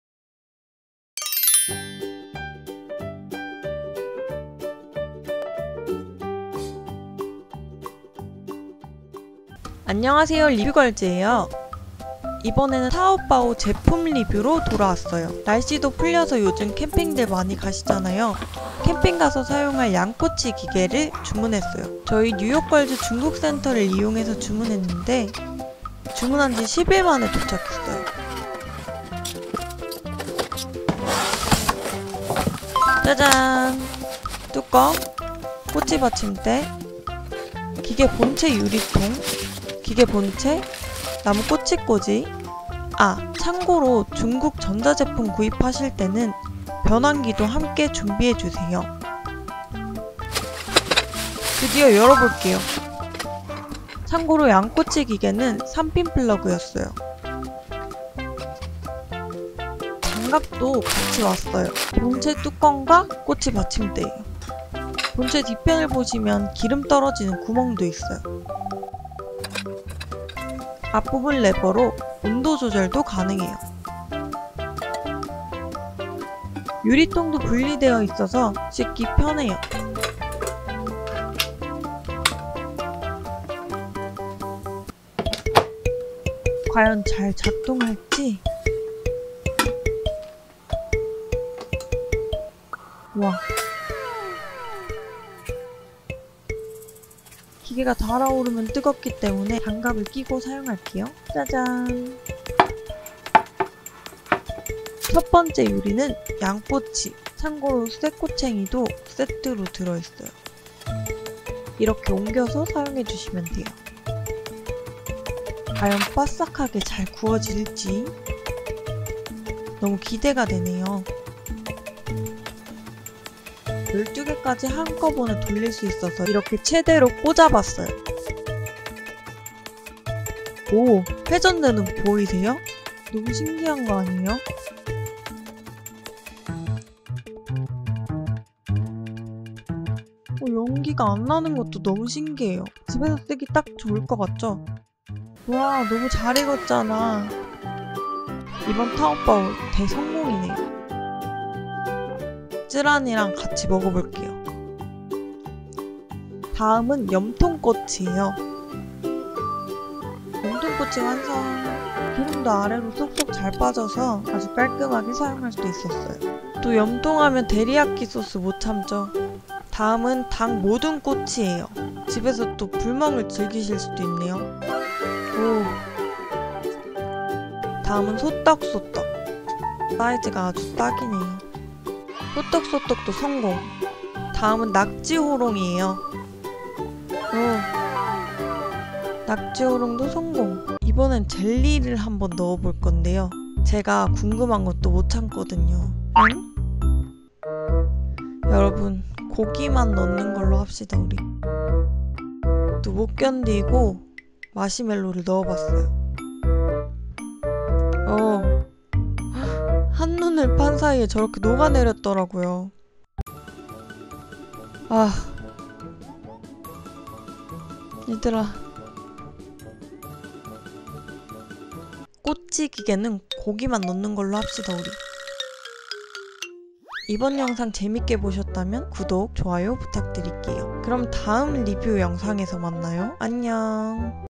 [ 리뷰걸즈 ] 양꼬치 기계는 양꼬치만 넣으세요...제발....🙏 | 타오바오 언박싱
안녕하세요
리뷰걸즈예요 !
이번에는 타오바오에서 구매한 양꼬치 기계 영상으로 돌아왔어요 !
영상 재밌게 봐주세요 : )

배송대행지 [뉴욕걸즈]
추천인 아이디 : misa9424